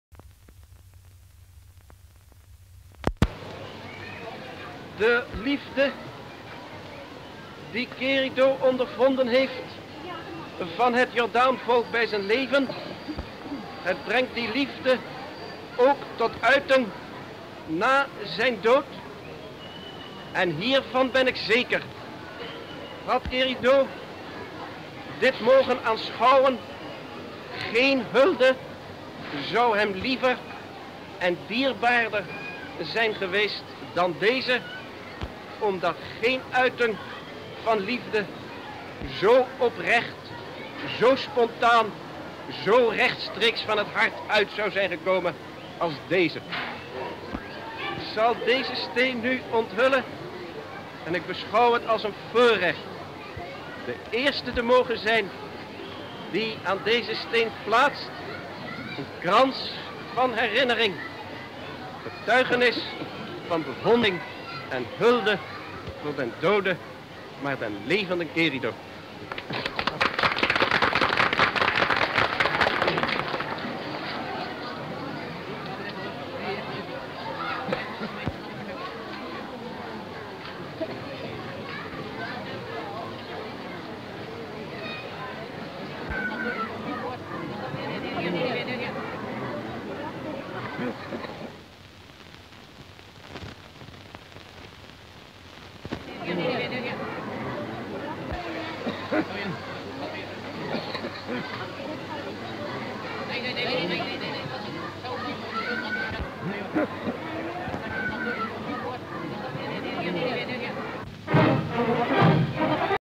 ALDERMAN BOEKMAN UNVEILS THE QUERIDO MEMORIAL AT THE JORDAN (Weeknummer 32) 🔴
Description (more): The unveiling of a memorial stone by Alderman Boekman, commemorating the Dutch novelist Israel Querido (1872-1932), in the Jordaan in Amsterdam. Shots:-div. shots of audience in Street and window openings while speaker, Alderman Boekman, (on increase, for microphone) speech in memory of the writer Israel Querido, after which he reveals stone façade 1 ' 22 "; -wall stone with inscription: "a group of Jordanian placed this stone in memory of Israel Querido friends that here his work" de Jordaan "created-1 october 1932" 5 "3"; -medium shot Querido to table 3 "; -wreath is hung over stone 12 ";